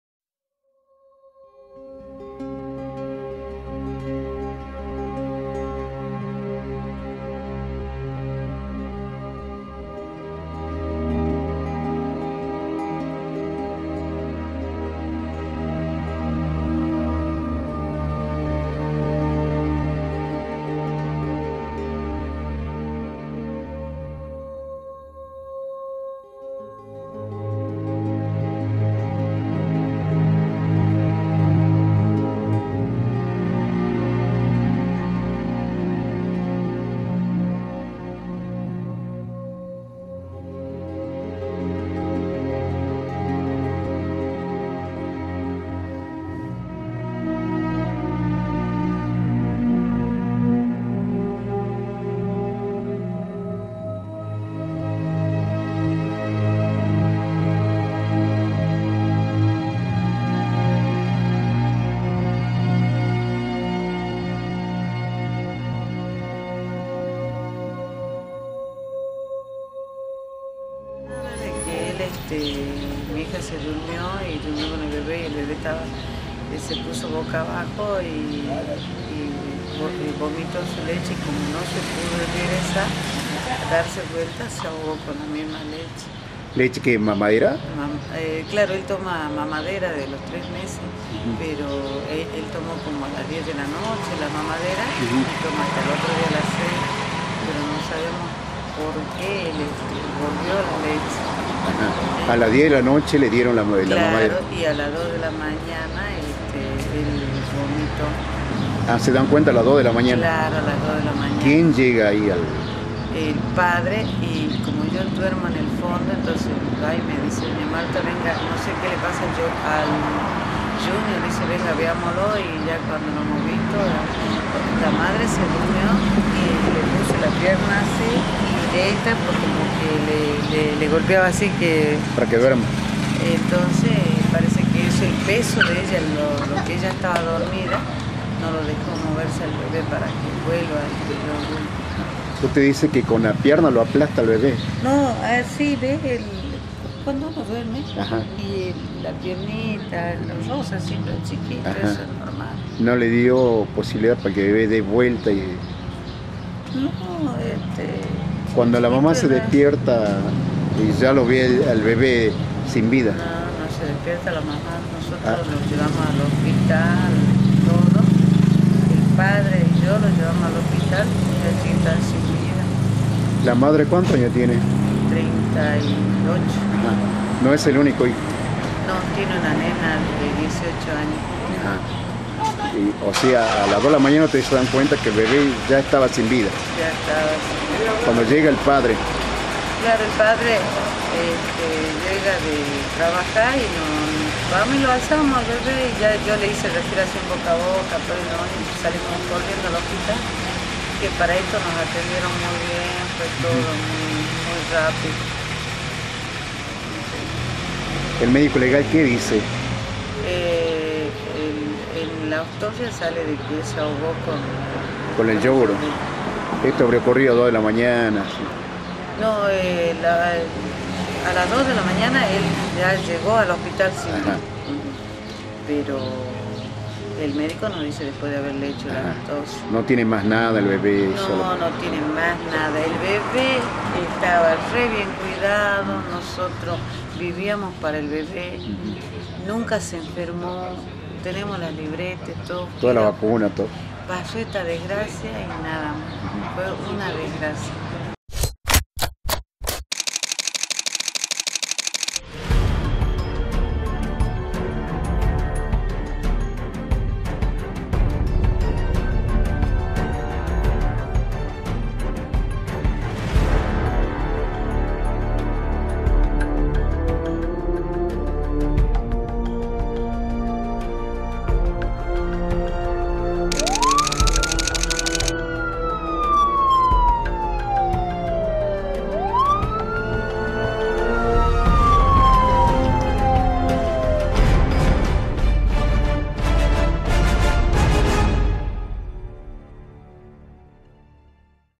INVESTIGAN LA MUERTE DE UN BEBE
El fiscal Cabot tomo intervención en la muerte de un bebe de 9 meses que llego sin signos vitales a la guardia del nosocomio local en las primeras horas de la madrugada. La madre fue demorada hasta que el informe del médico forense determino que el deceso fue a consecuencia de una bronco aspiración. La abuela materna dijo en el programa “El Margen” que el lactante estaba tomando una mamadera cuando se produjo el desenlace fatal.